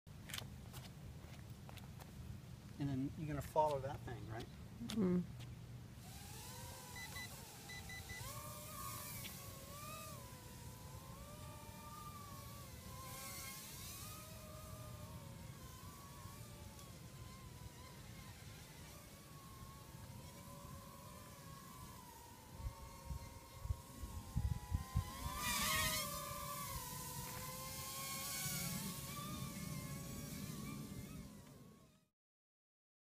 Quick Video with Nano-Drone
Here's a quick video with my nano-drone at Aliso and Woods Canyons.